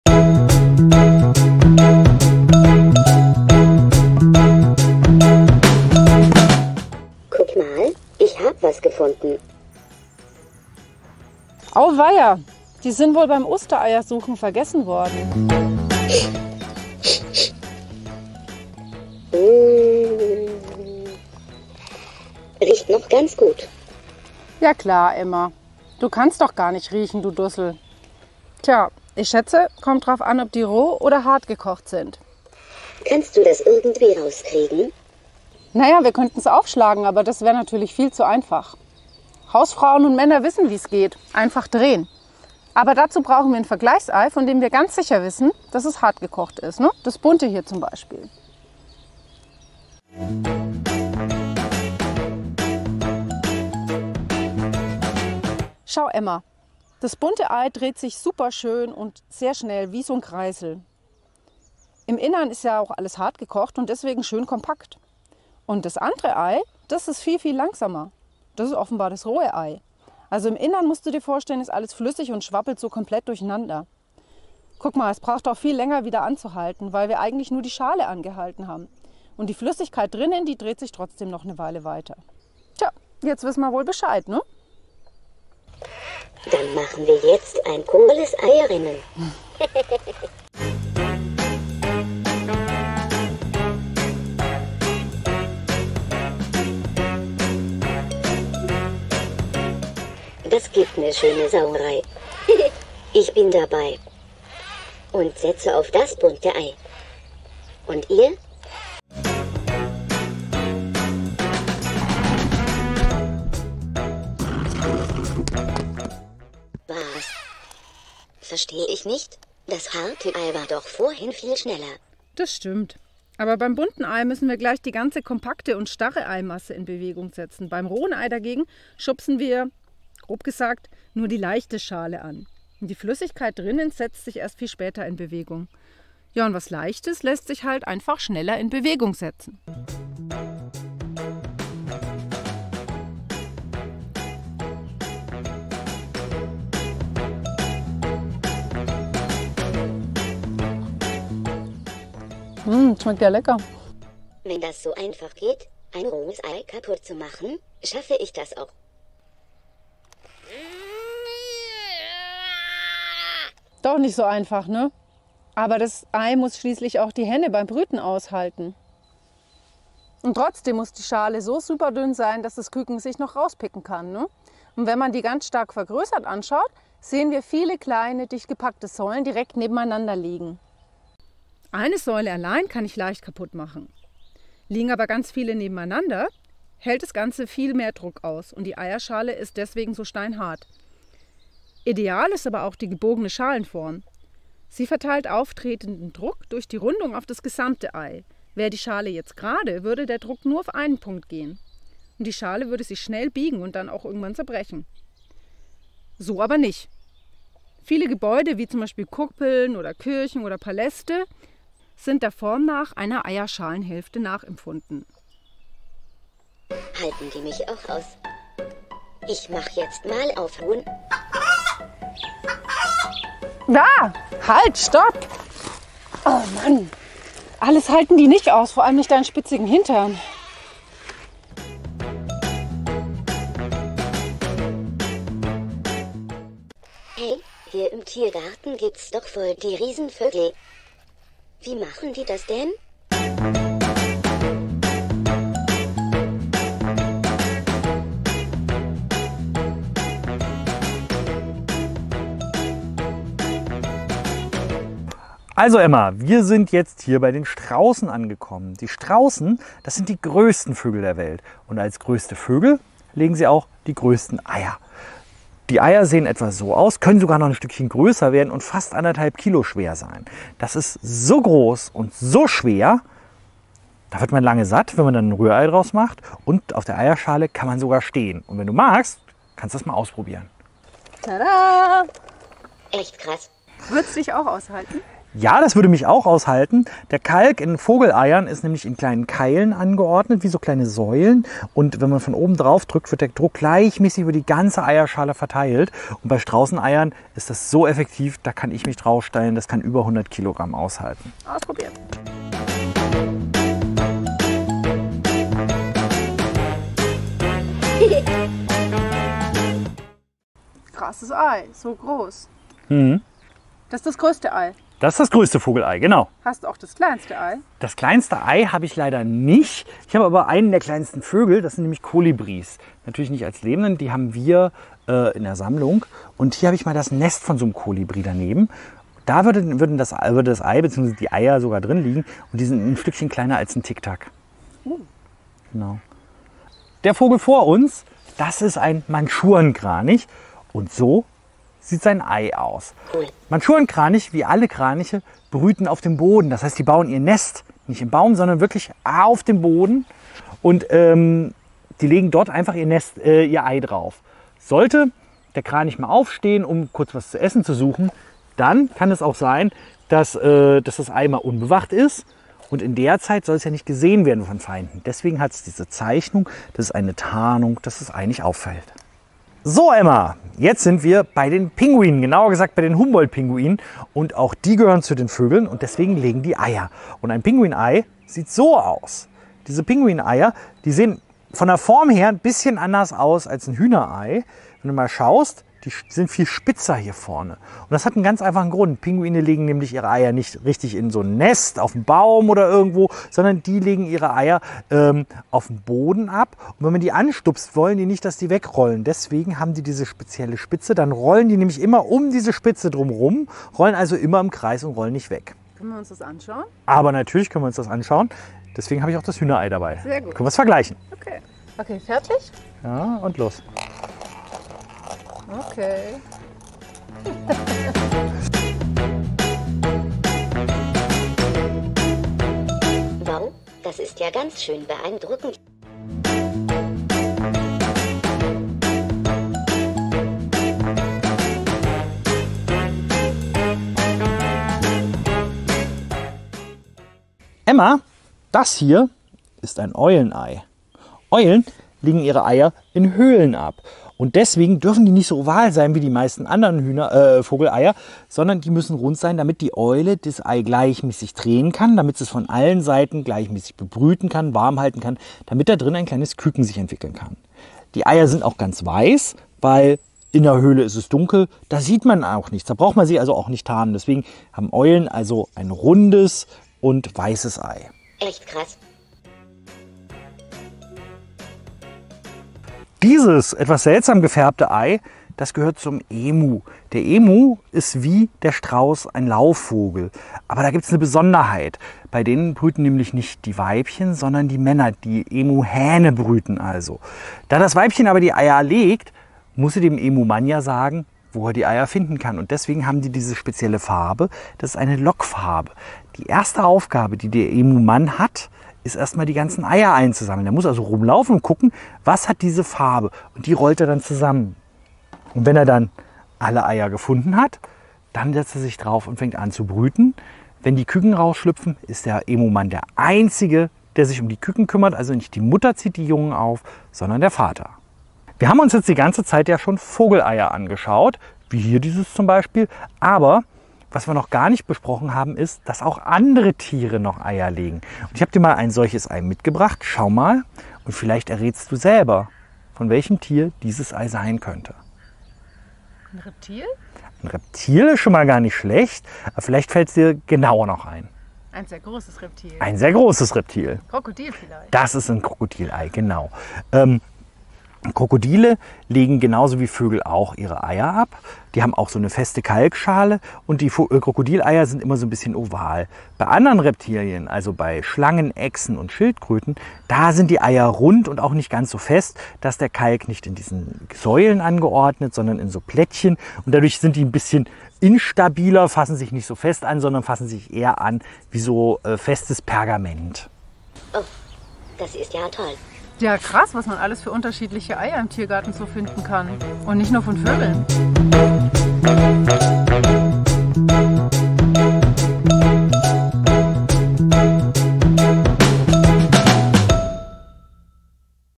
Roboter Emma auf Eiersuche - Bionik auf der Spur
Roboter Emma entdeckt ein Ei im Tiergarten. Ist es ein Straußenei oder hat es jemand bei der Ostereiersuche vergessen? Mit Alex vom Bionicum und Chris vom Tiergarten begibt sich Emma auf eine Eiersuche im Tiergarten.